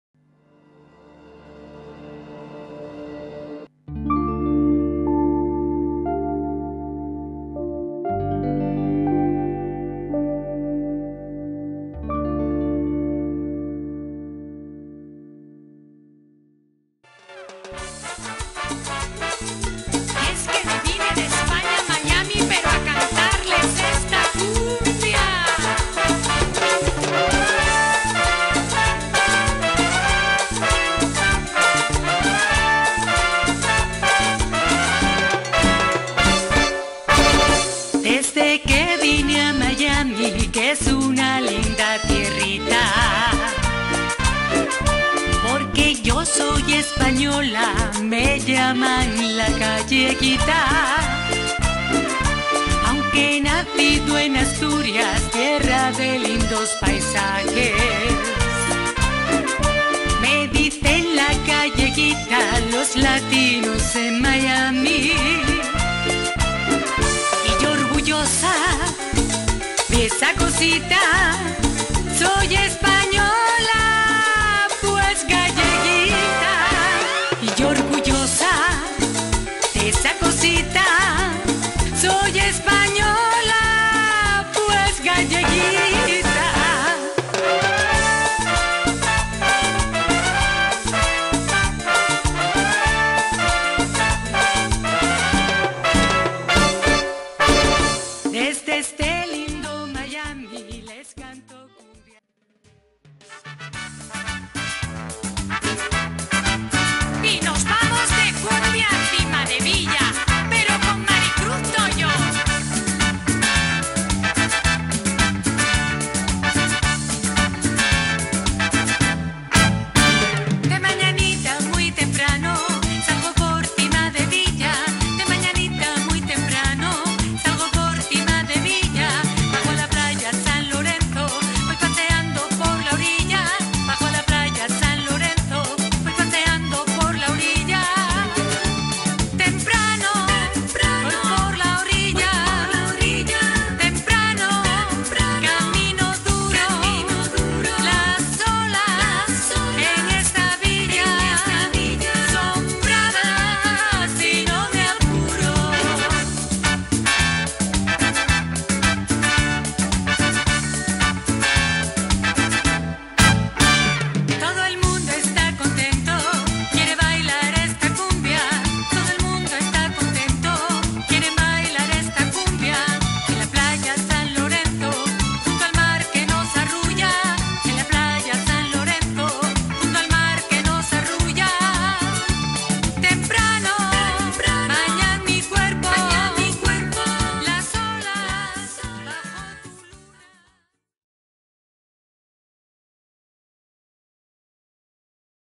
MARY CRUZ TOYOS / AGRUPACIÓN ARTÍSTICO GIJONESA [Noches en la Plaza Mayor] - Gijón
NOCHES EN LA PLAZA MAYOR (Gijón) / miércoles 07 - 21:00h

Mary Cruz Toyos y La Agrupación Artisítca Gijónesa desde las 21 horas en Noches en la Plaza Mayor.